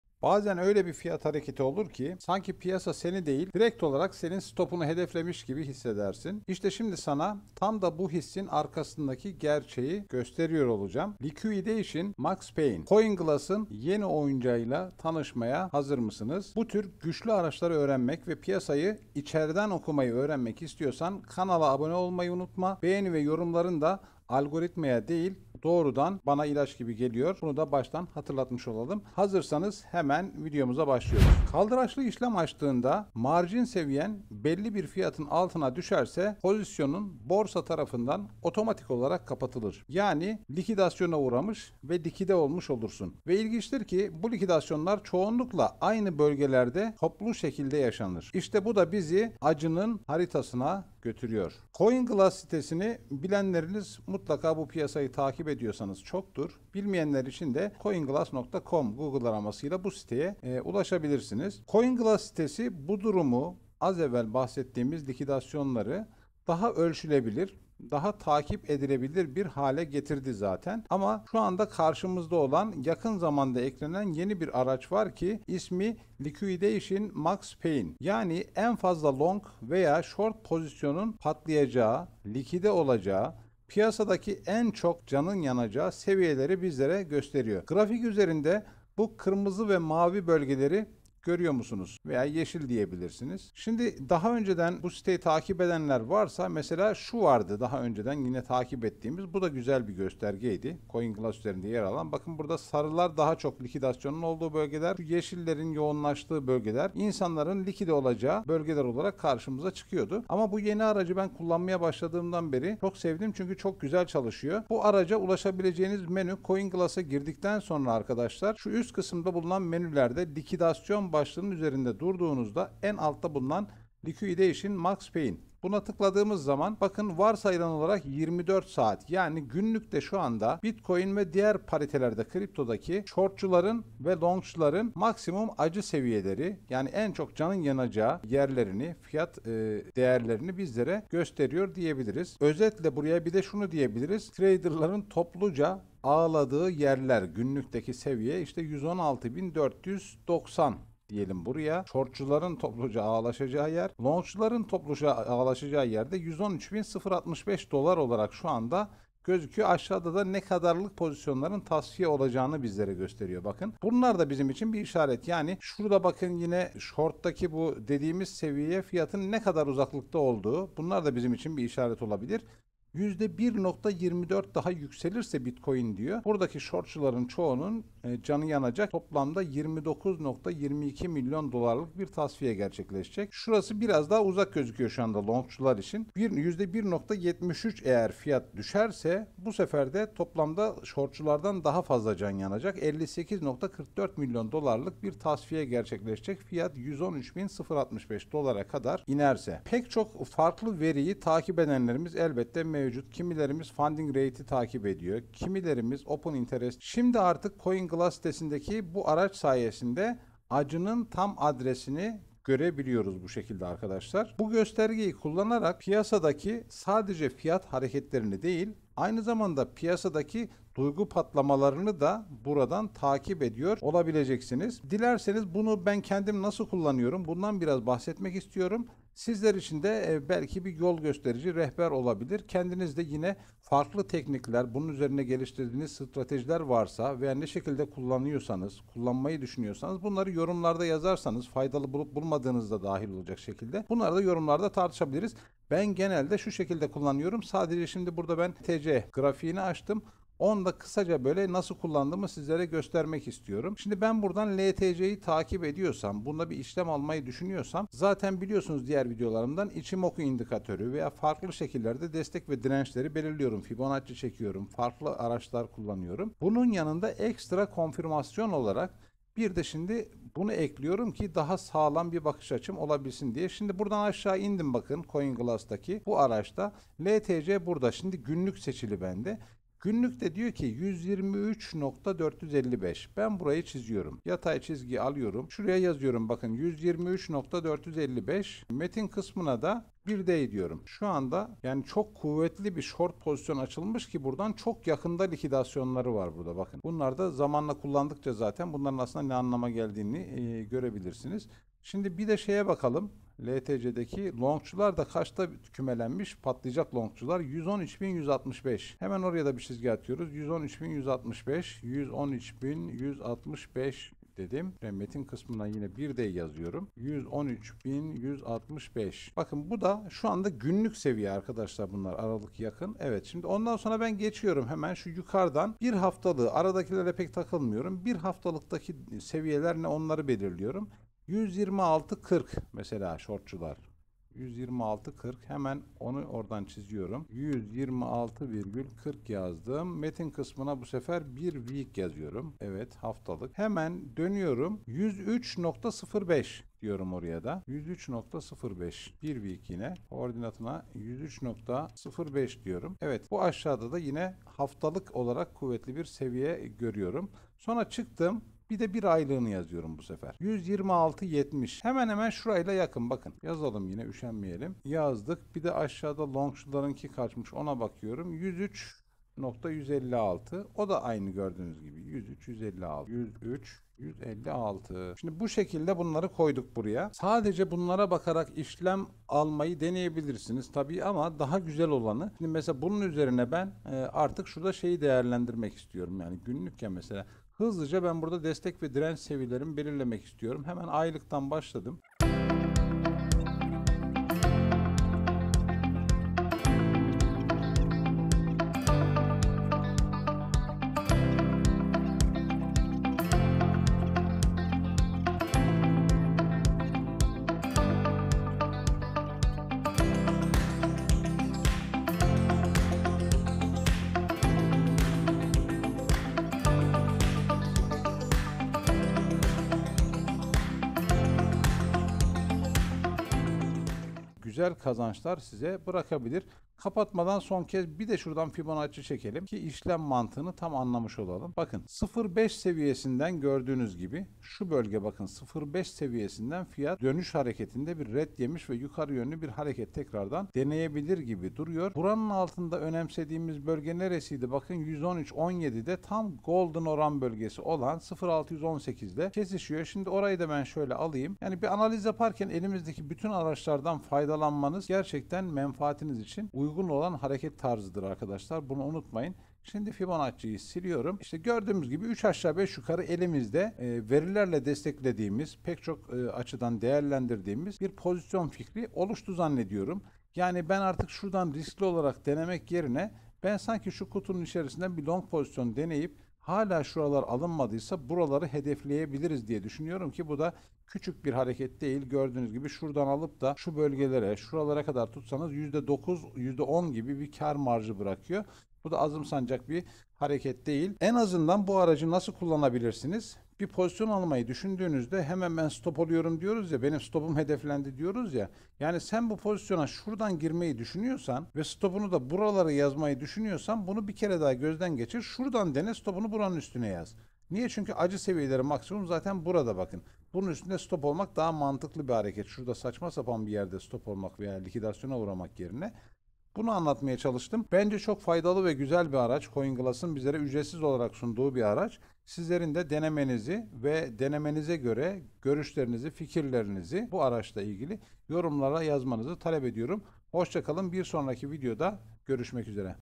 CoinGlass'ın Yeni Oyuncağı Liquidation Max Pain Nasıl Kullanılır?
How to Use CoinGlass’s Liquidation Max Pain Indicator (Trader’s Secret Tool!)
📊 CoinGlass’ın yeni aracı “Liquidation Max Pain” nedir, nasıl çalışır ve bu veriyi kullanarak piyasada nasıl avantaj sağlarsınız? Bu videoda hem kısa bir likidasyon özeti hem de bu güçlü indikatörün ekran üzerinden anlatımı var.

📈 Bu göstergeyle, toplu likidasyonların yaşandığı fiyat bölgelerini görebilir ve potansiyel kırılma noktalarını önceden fark edebilirsiniz.

💡 Strateji anlatımı ve detaylı rehberler için yorumlarda belirtmeyi unutmayın.

📊 What is CoinGlass’s new “Liquidation Max Pain” indicator, how does it work, and how can traders use it to their advantage?

In this video, you’ll get a quick overview of liquidation mechanics and a hands-on guide to reading the Max Pain data on-screen.

Spot key levels where most traders get liquidated and use that insight to avoid becoming one of them.